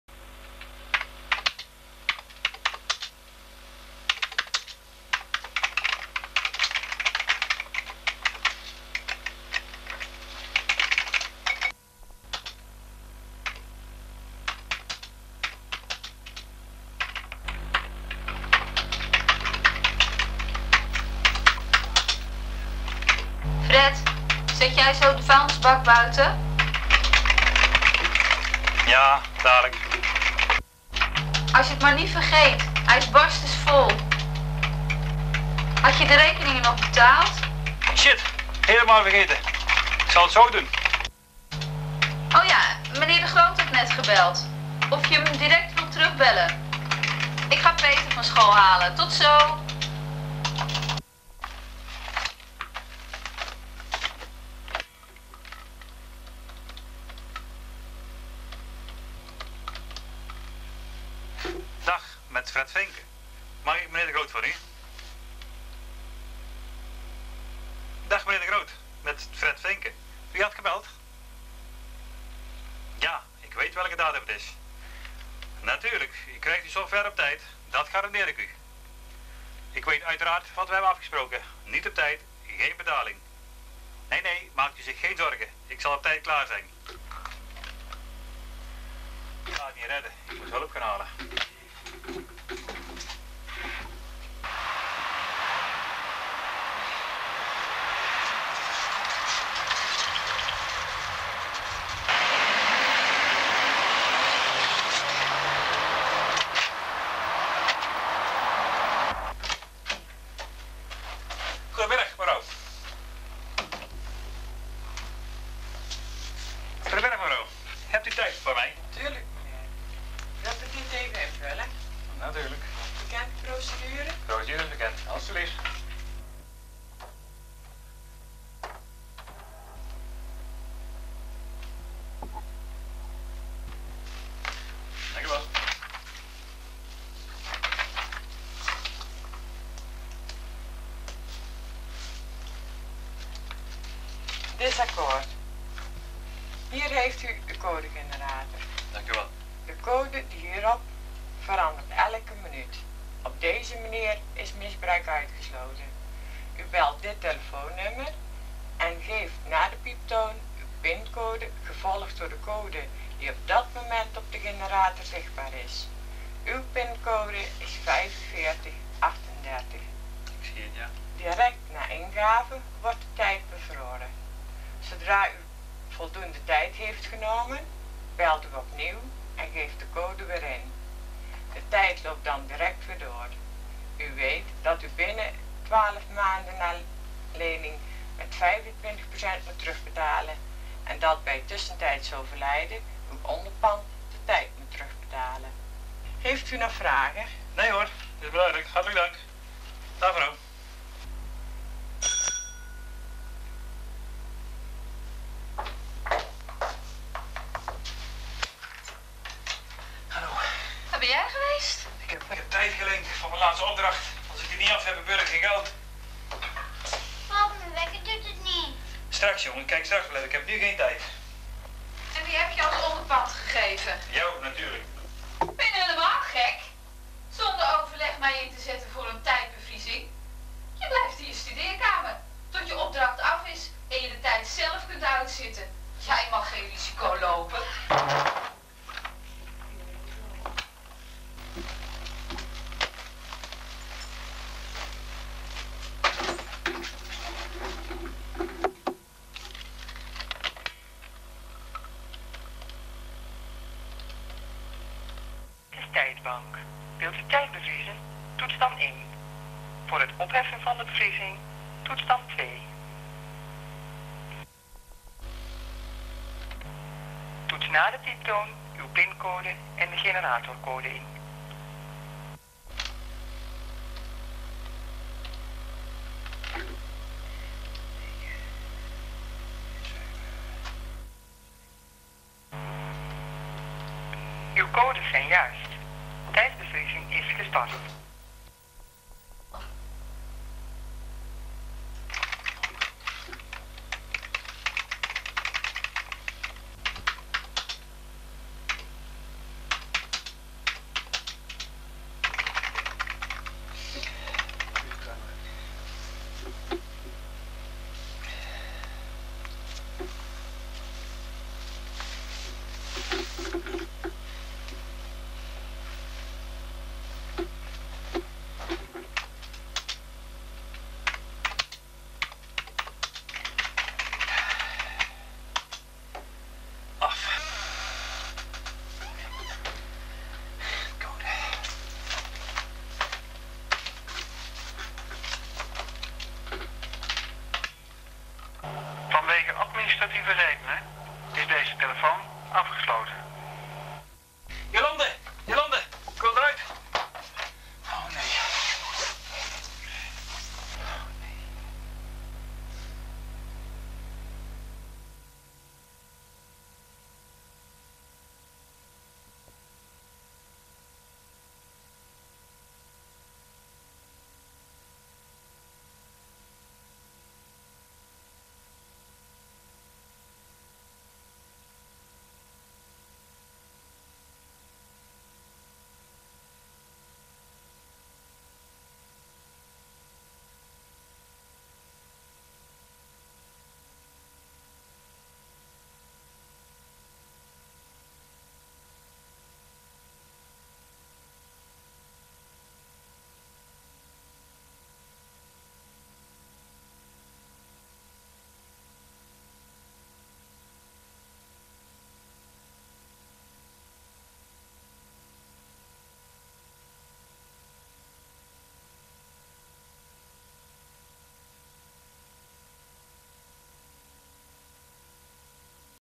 Relativiteit Realitijd
Een programmeur moet voor het nieuwe millennium zijn werk klaar hebben, heeft hij genoeg tijd?
Een Videofilmclub OASE film uit 1998.